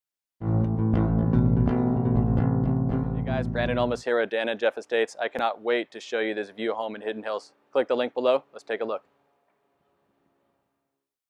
Branden Intro - 24835 JACOB HAMBLIN, HIDDEN HILLS
Serene Cul De Sac Panoramic View Estate

7 Bed | 5.5 Bath | Price Upon Request

Privacy and compelling panoramic jetliner views embody this immaculate and charming traditional. Set behind its own security gates at the end of a secluded cul de sac this 7 bd 5.5 bath boasts all the amenities to enjoy the ultimate for indoor/outdoor living. Immaculate throughout with designer upgrades, newly painted and carpeted. Double doors open to a comfortable Living Room with soaring ceilings, wood floors and warming fireplace. Spacious Family Room highlighted by a custom fireplace, wet bar and built ins is served by a fully amenitized Center Island country kitchen with exposed beamed ceilings, walk in pantry & generous sun filled breakfast area. A separate staircase leads to the romantic Master Suite with spacious walk in closet, sunlit bath with double sinks, spa tub and glorious panoramic views. Lushly landscaped exterior grounds w/ sparkling pool, spa, BBQ center & stone patios. Breathtaking city light & mountain views beyond.

Listing by Compass
Dana Olmes & Jeff Biebuyck